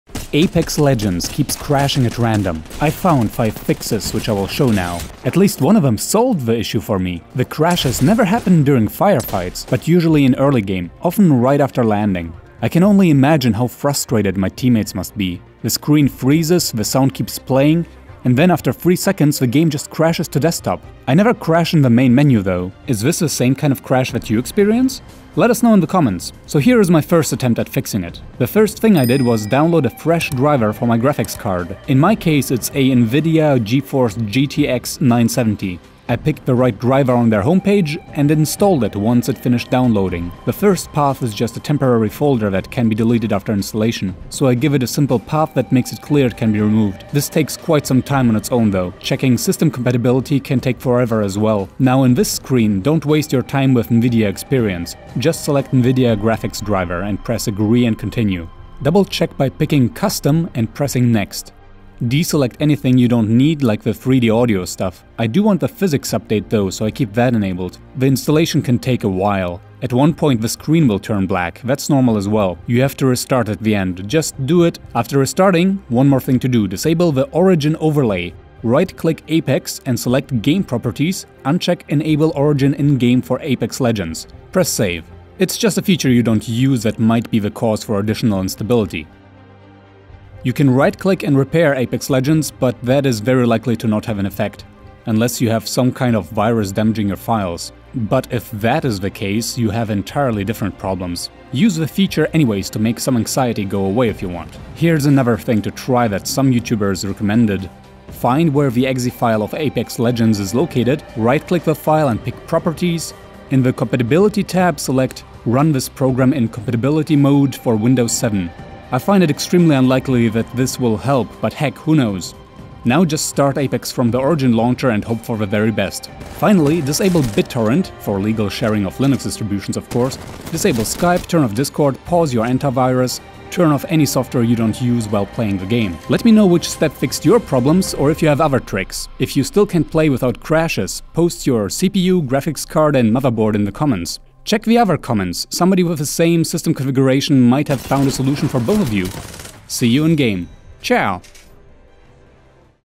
Apex Legends Keeps Crashing (Fix)
Apex Legends kept freezing, then crashing so I fixed it (5 things to try)

iwanPlays - I make videos about Ravenfield, modding, and indie games.